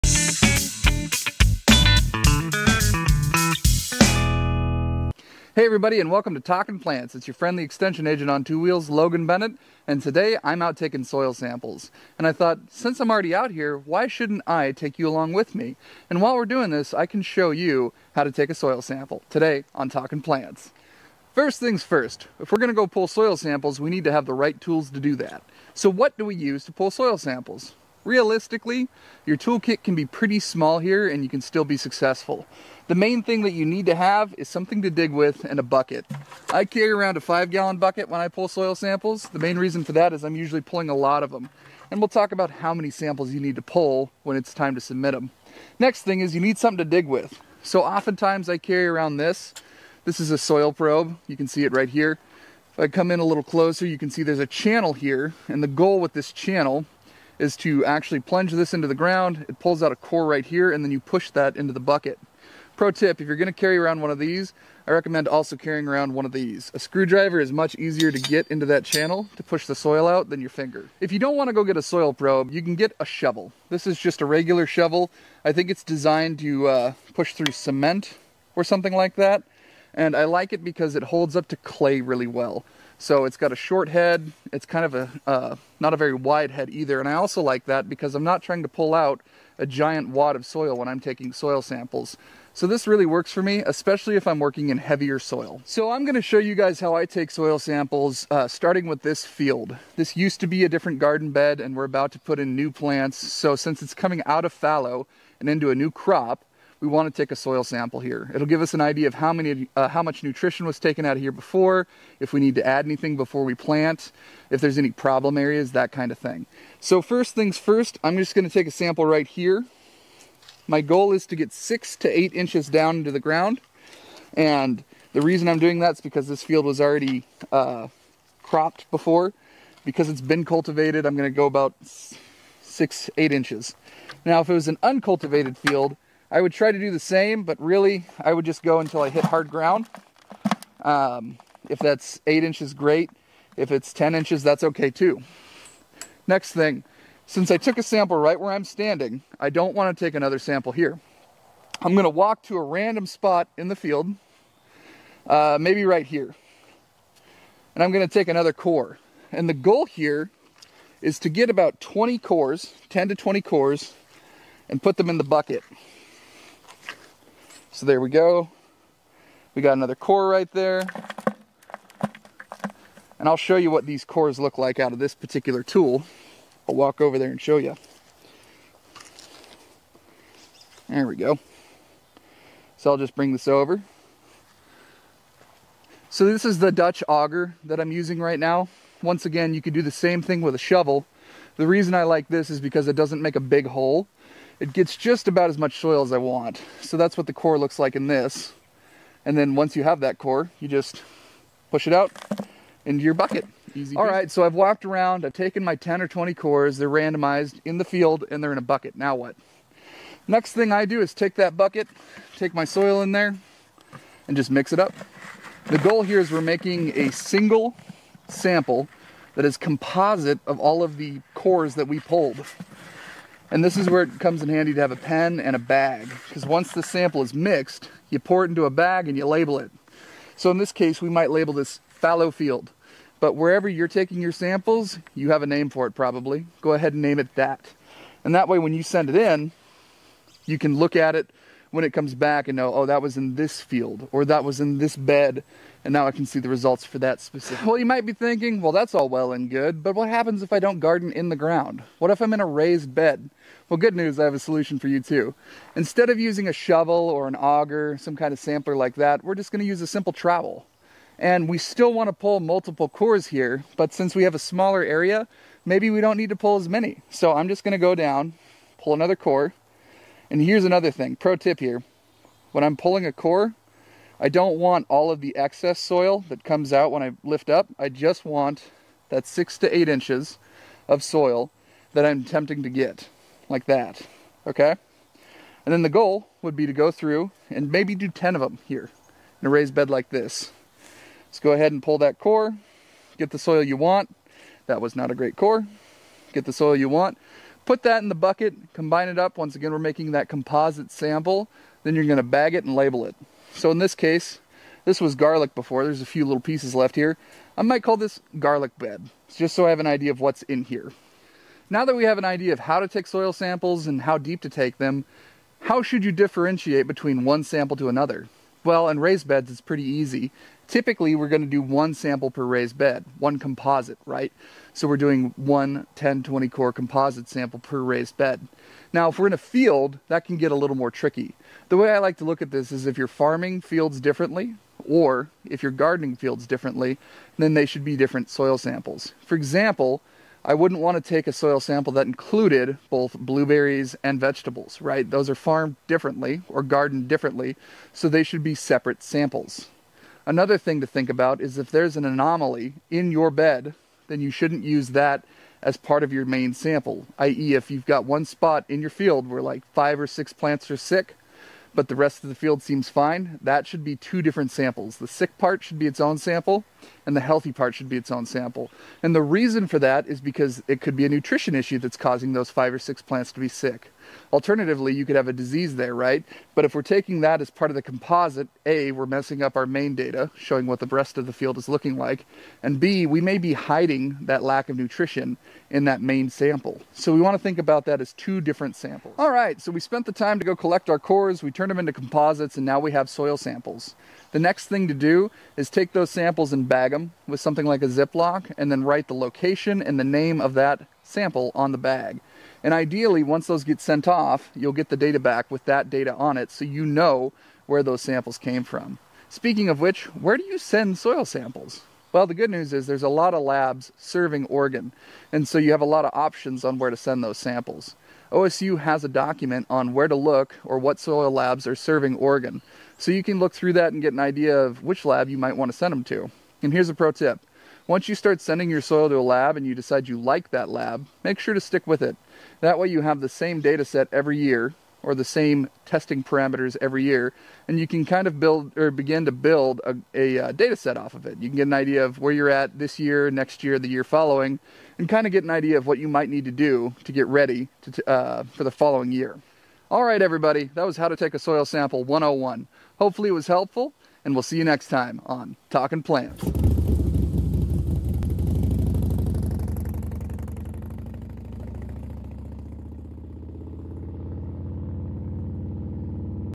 How to take a soil sample 101
Come along with Logan and learn how to take soil samples from your garden or field beds.